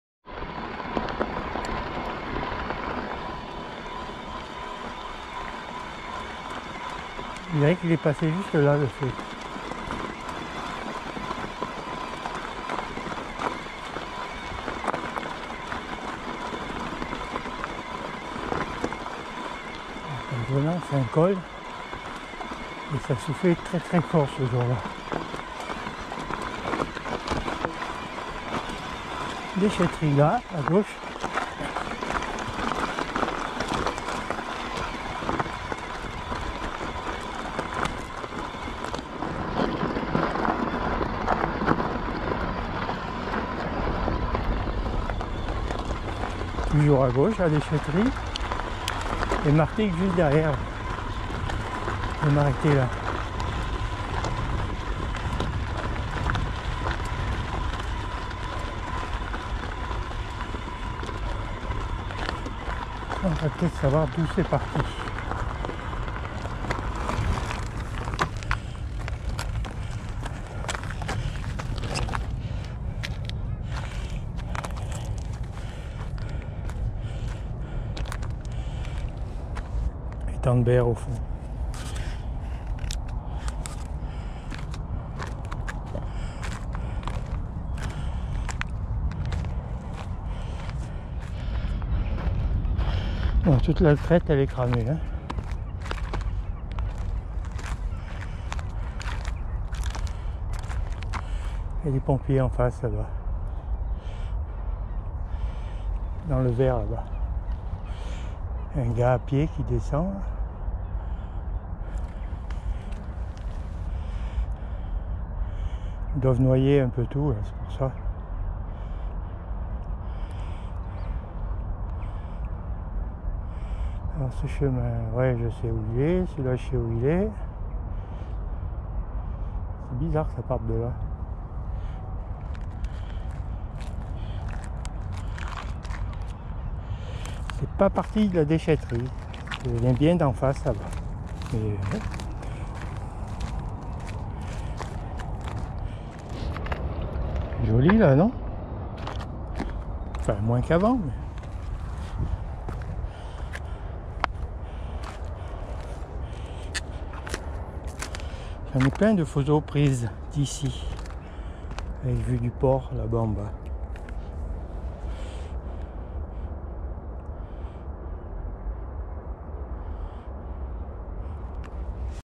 incendies martigues 04 aout 2020 part 5
tour d'horizon des incendies du 04 aout 2020 sur martigues quartiers des ventrons , des reveillés et les moulins en passant par le vallon du fou.